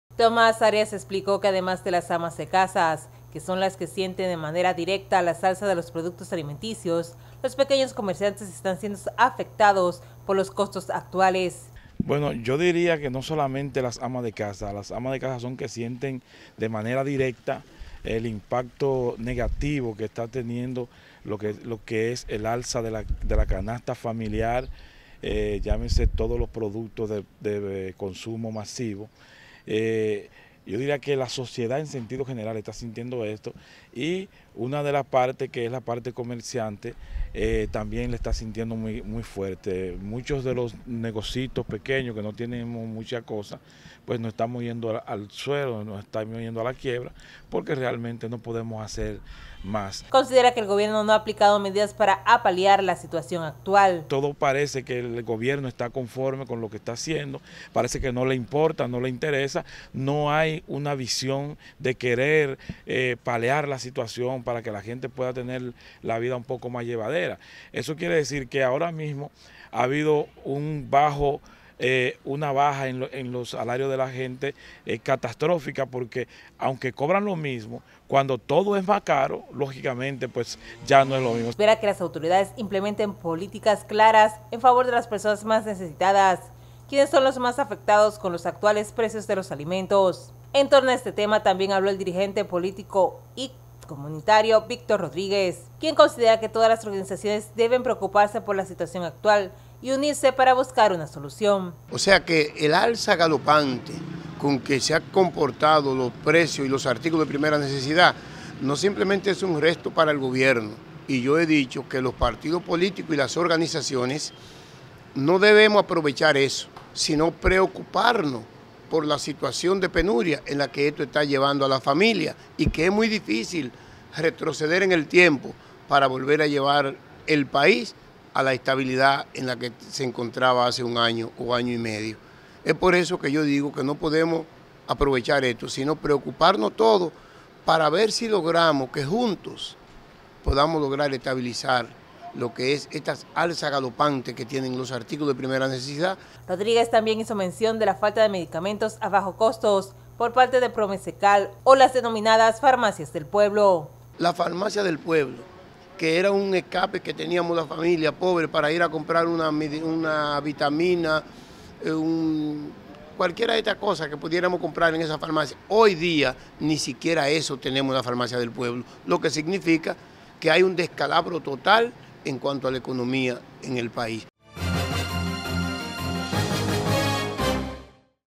Dirigentes comunitarios hablan sobre altos costos de los alimentos
El dirigente comunitario del sector  Brisa del Norte, Tomás Arias, señaló que la sociedad en sentido general   está  siendo perjudicada con el alza de los productos de la canasta básica.